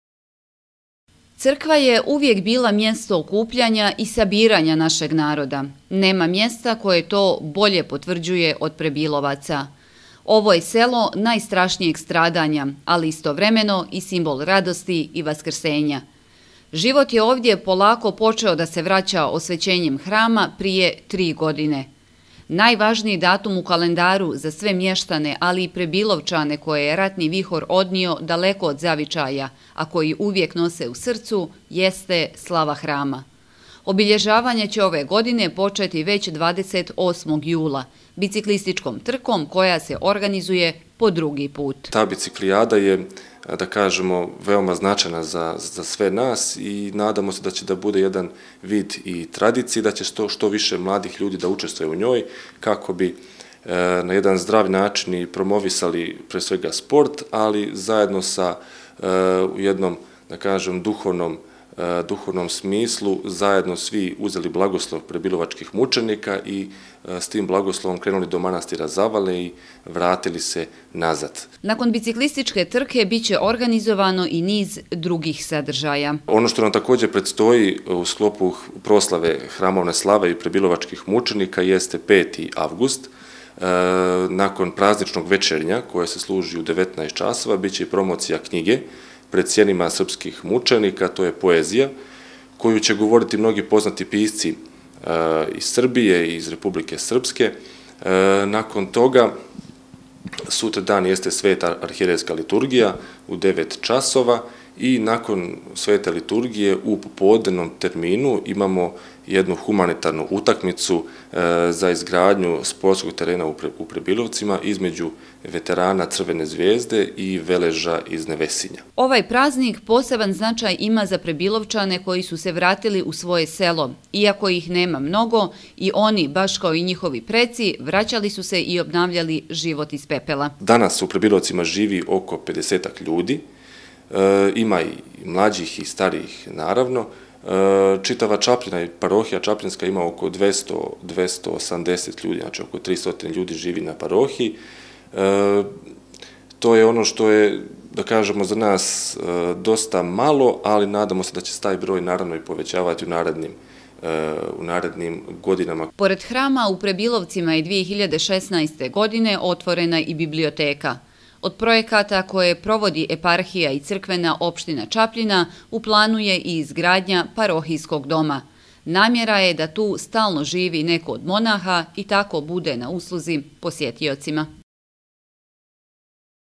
Prebilovci: Nizom aktivnosti počinje obilježavanje slave Hrama (27.07.2018.)
U Prebilovcima su u toku pripreme za slavu Hrama, Svete mučenike prebilovačke i donjohercegovačke, koji se obilježavaju 6. avgusta. Nizom kulturnih i sportskih dešavanja, kao i crkvenim obredom biće proslavljen ovaj najvažniji dan. Sa obnovom i osvećenjem Hrama, počela je i obnova života u ovom stradalnom srpskom selu.